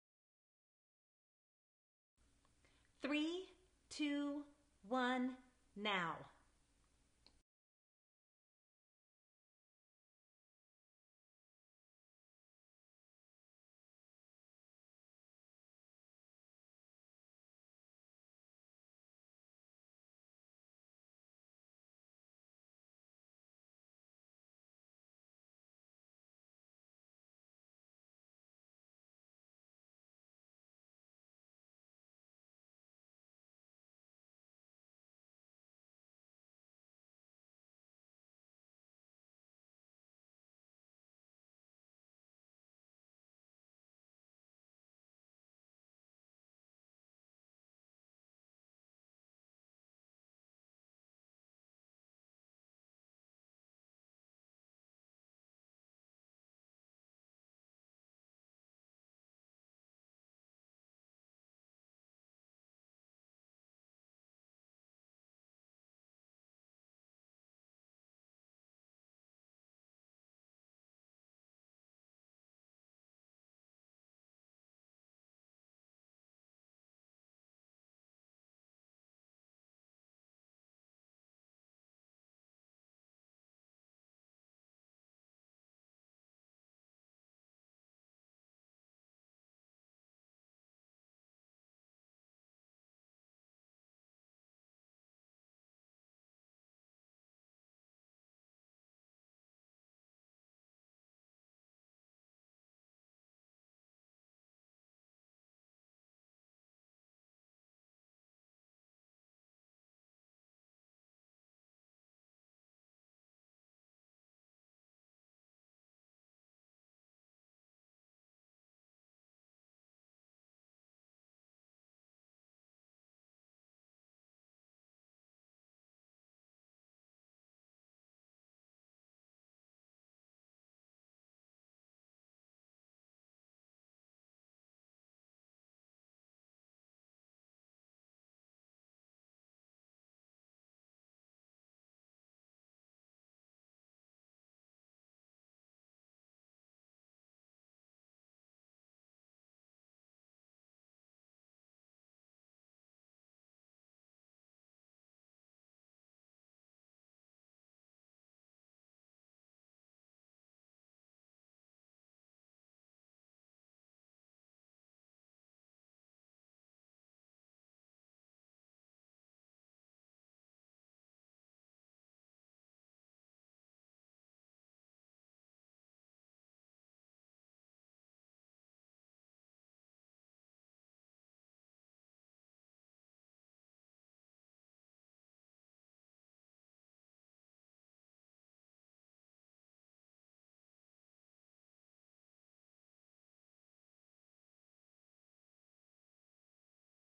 SL.mov
A revolutionary interval-style dance class powered by specifically choreographed popular music. A fusion of dance styles offers something for everyone.  Phusia - Shaping women into dancers.  www,phusia.com